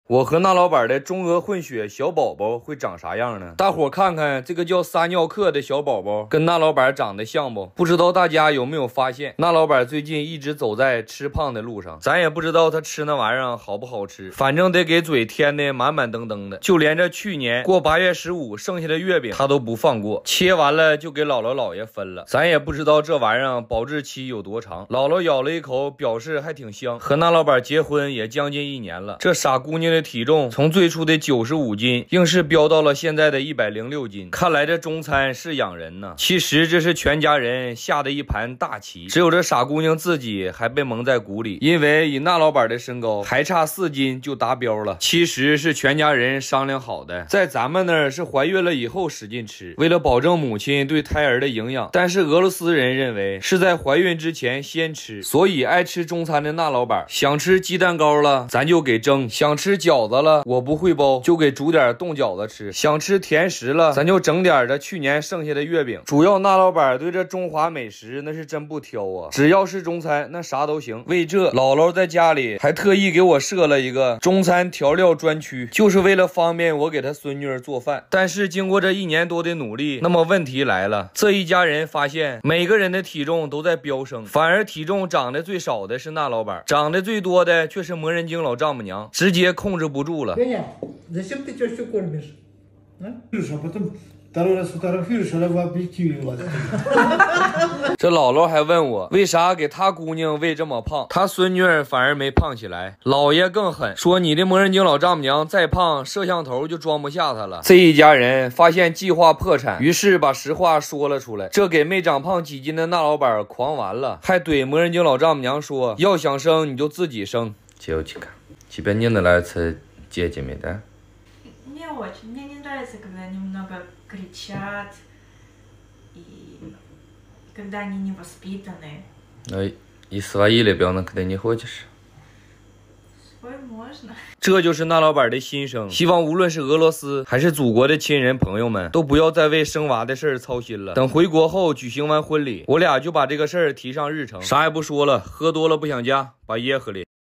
我和俄罗斯媳妇的混血宝宝，到底会是长啥样的呢？混血宝宝计划提上日程【俄罗斯足球小子】
hello，大家好，这里是【俄罗斯足球小子】YouTube唯一频道。
我会在这里分享我的日常，希望大家能够喜欢~
我和俄罗斯媳妇的混血宝宝，到底会是长啥样的呢？混血宝宝计划提上日程【俄罗斯足球小子】 请放心，本视频内容符合YouTube社区准则，没有任何违反规定的行为。